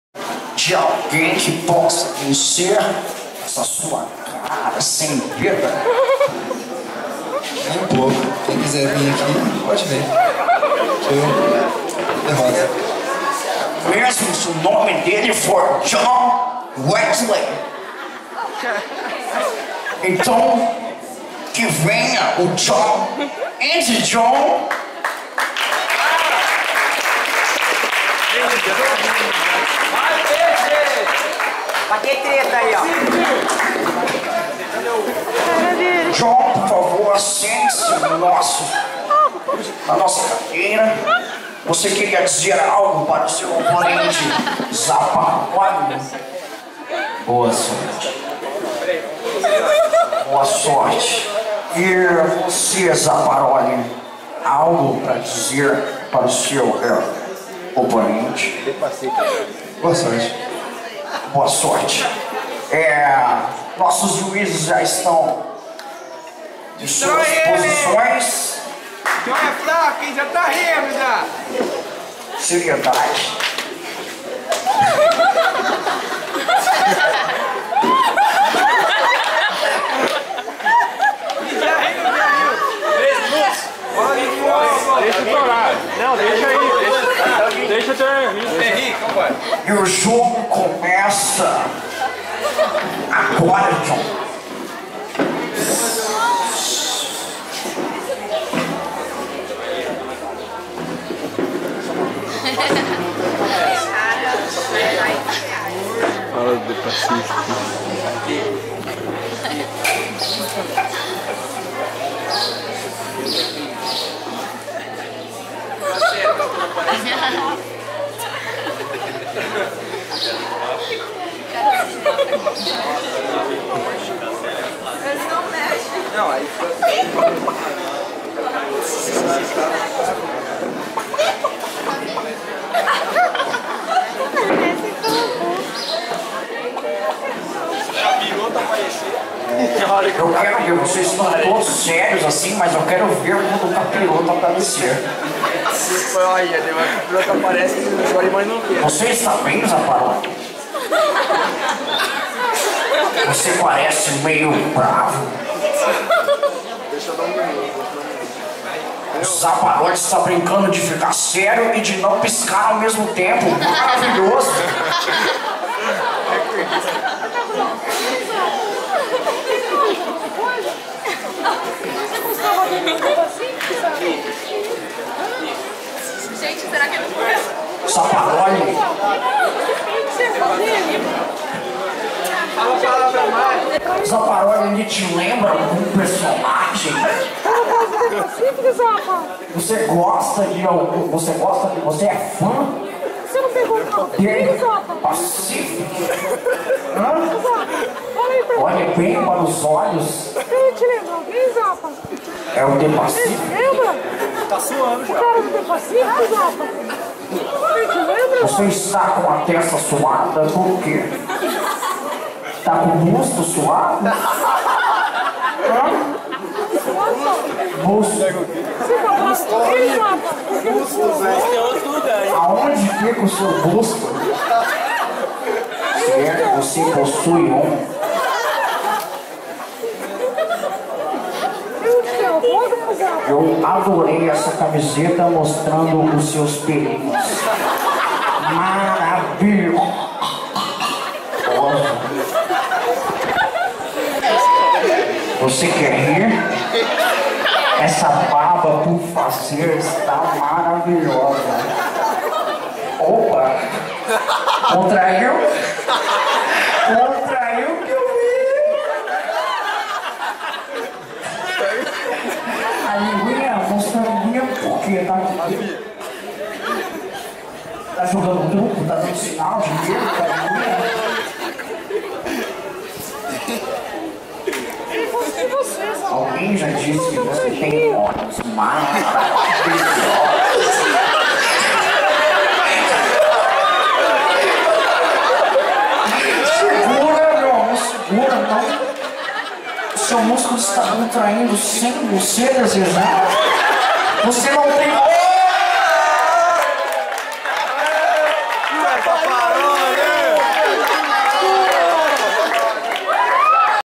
Jogo do Riso - John x Zapa
Brincadeira no último dia de Retiro Espiritual em São José do Rio Preto. Valeu pessoal por tantos momentos de alegria que passamos juntos! Dias de muitas bençãos de Deus.